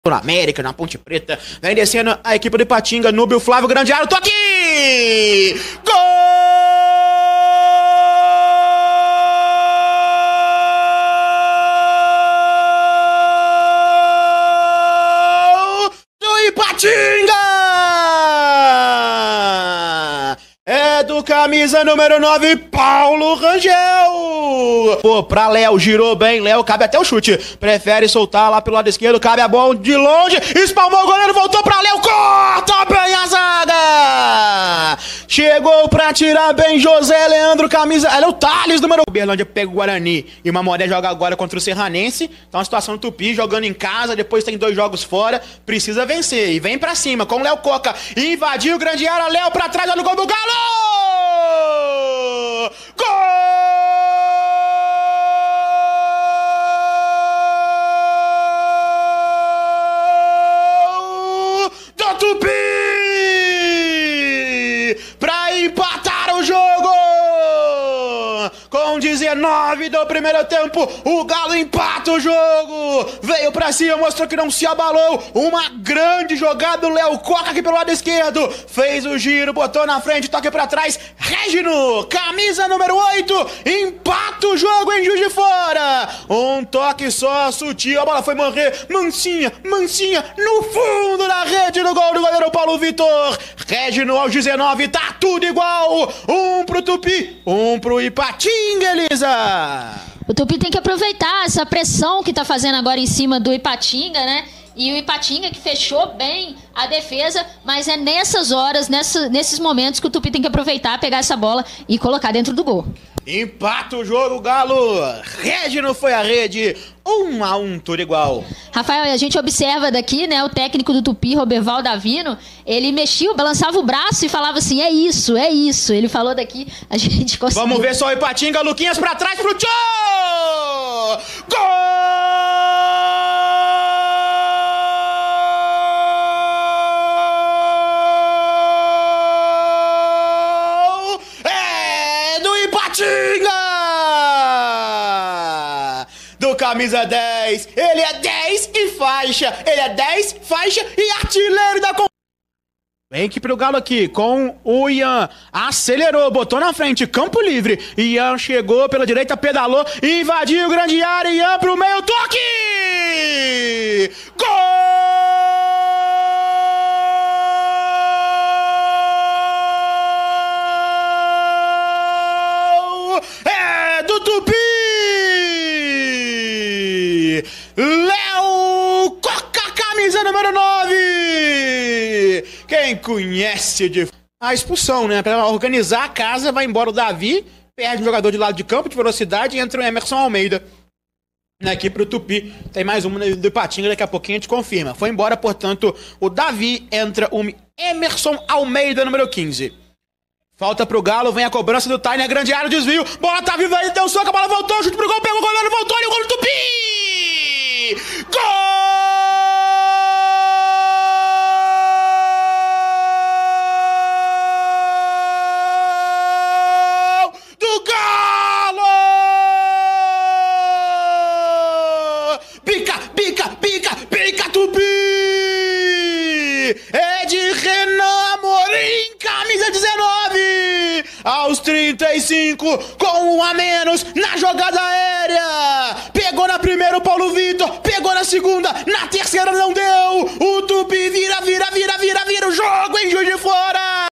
Campeonato Mineiro Módulo II: Tupi 3 x 2 Ipatinga - Melhores Momentos
Acompanhe os lances da vitória do Tupi sobre o Ipatinga pelo placar de 3 a 2, em jogo válido pela oitava rodada do Campeonato Mineiro Módulo II.

TV NSPORTS, SEU ESPORTE AO VIVO E ONLINE!